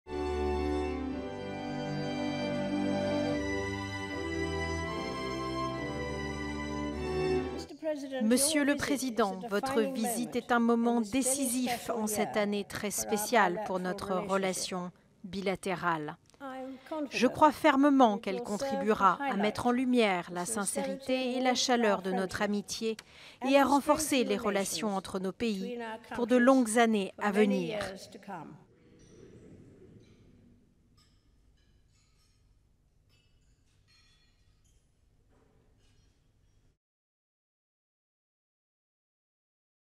Royaume-Uni: banquet royal à Londres pour Xi Jinping
Le président chinois Xi Jinping a entamé mardi 20 octobre en grande pompe une visite d'État de quatre jours au Royaume-Uni au cours de laquelle des accords portant sur plus de 30 milliards de livres (environ 40 milliards d'euros) doivent être signés. La journée s'est clôturée par un banquet officiel au palais de Buckingham donné par Elizabeth II. Durée: 00:40